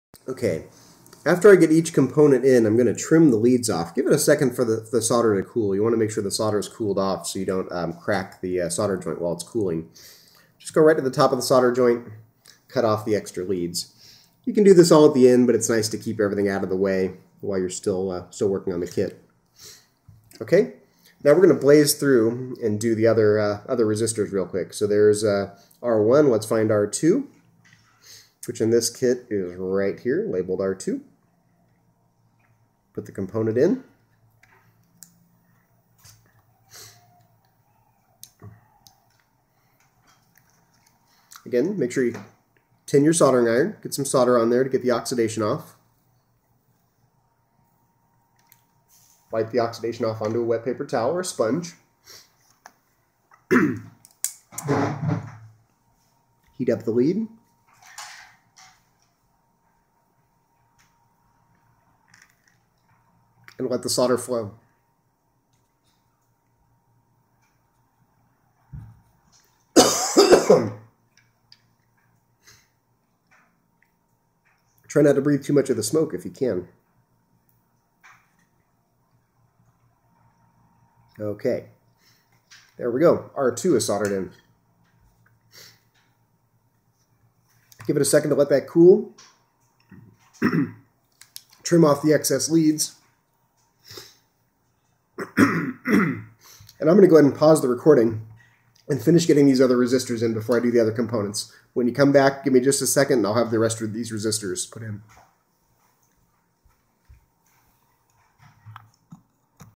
Part 2: Assembling the Velleman Flashing LED Sweethearts Electronic Kit
Zachary Fruhling Assembling the Velleman Flashing LED Sweethearts Electronic Kit